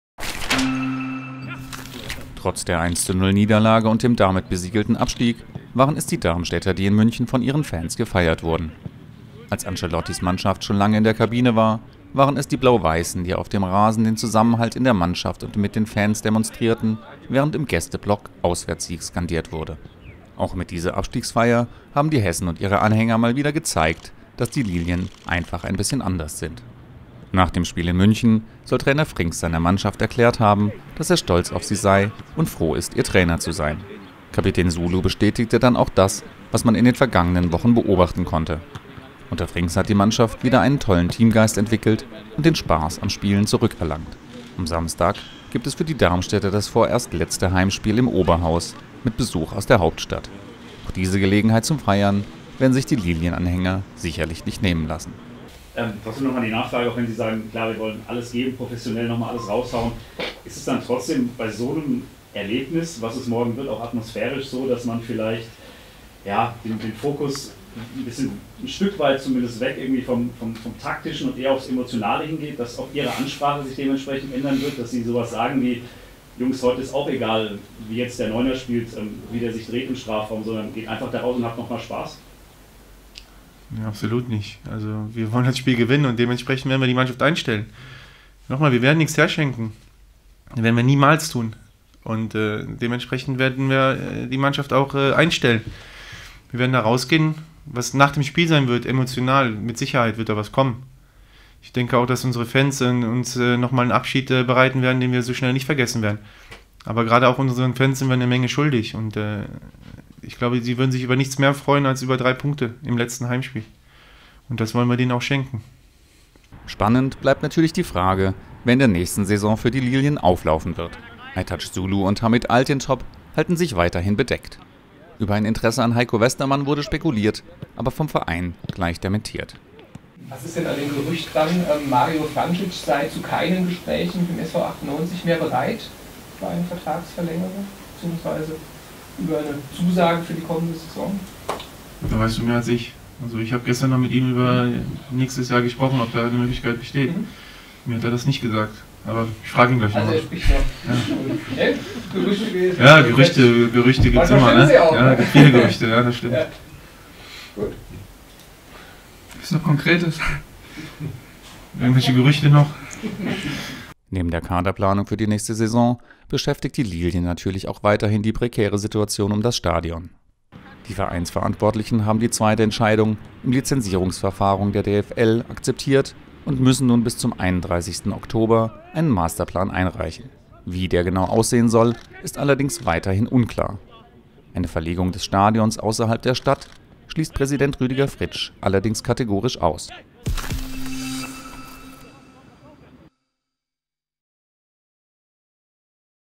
Heimspiel des SV Darmstadt 98 gegen Hertha BSC
Am Samstag spielen die Lilien das vorerst letzte Heimspiel in der Ersten Bundesliga. "Wir werden nichts herschenken", sagt Trainer Torsten Frings.